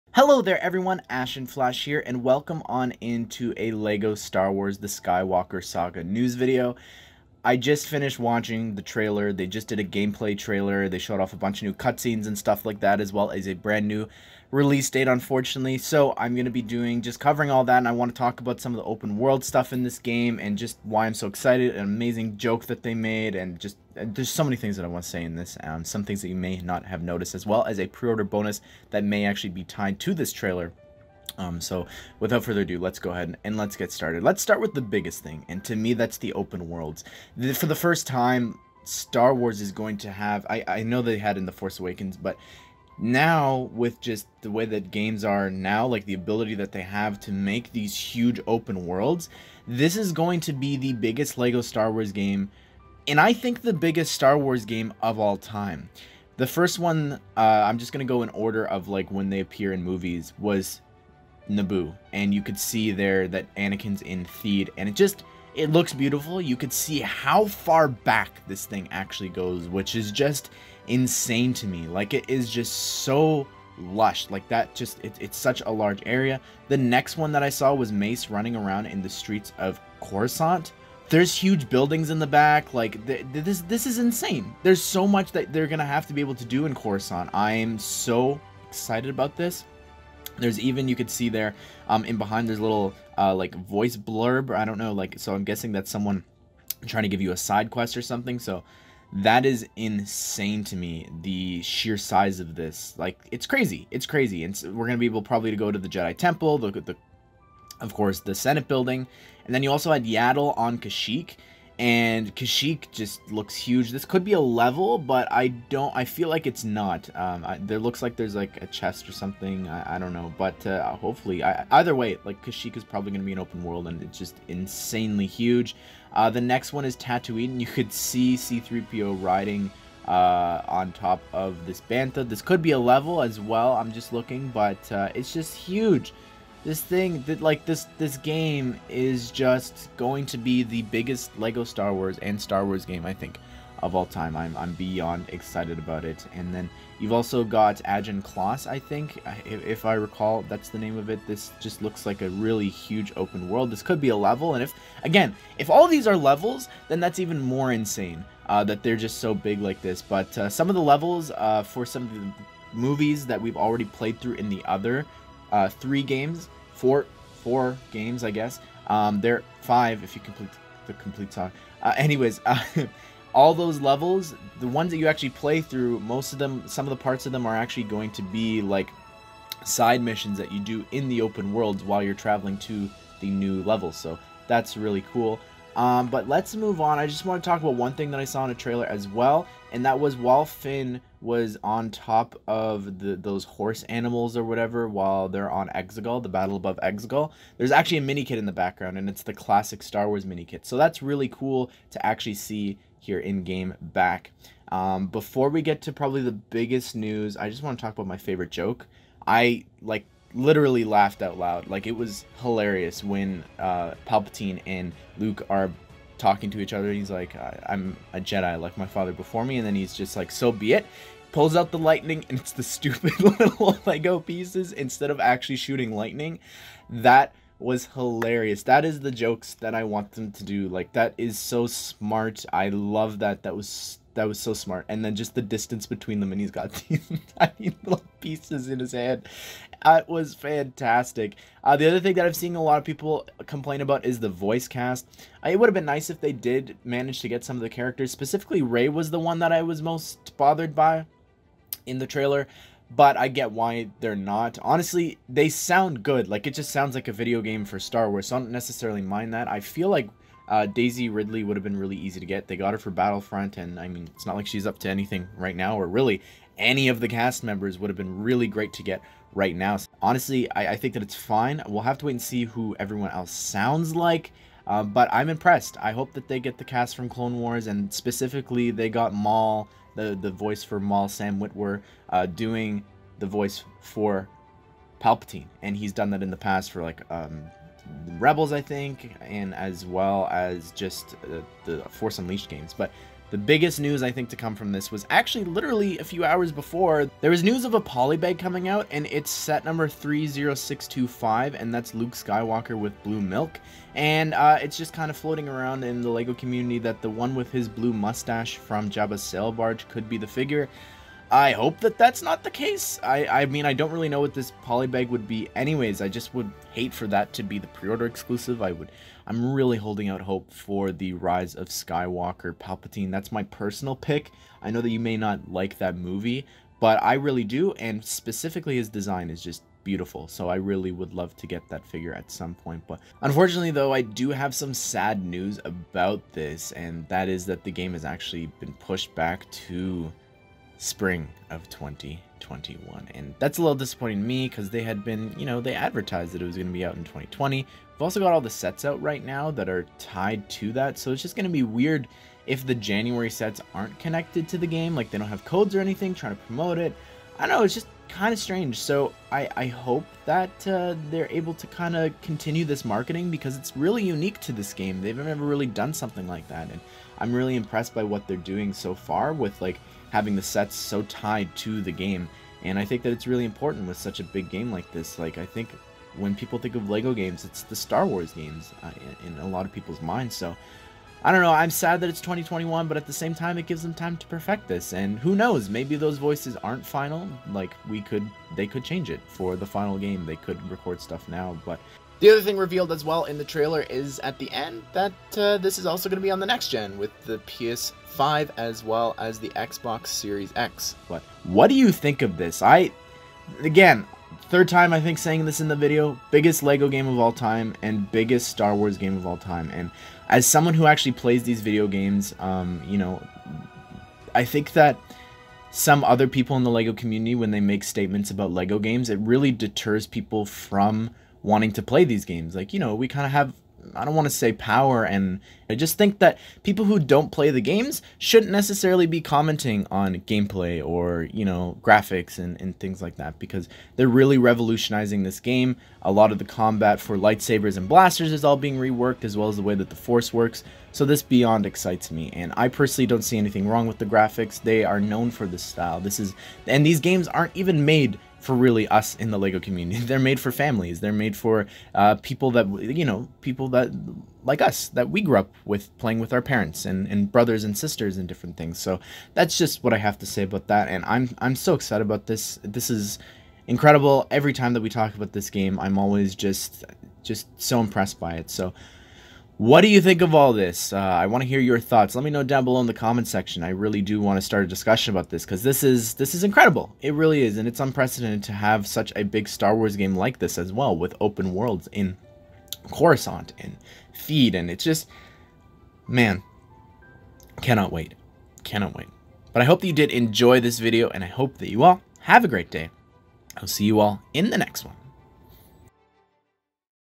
LEGO Star Wars Skywalker Saga Trailer Breakdown - Pre-Order Exclusive Figure Confirmed
Today, we're breaking down the latest trailer for LEGO Star Wars the Skywalker Saga! My goodness this game is going to be amazing!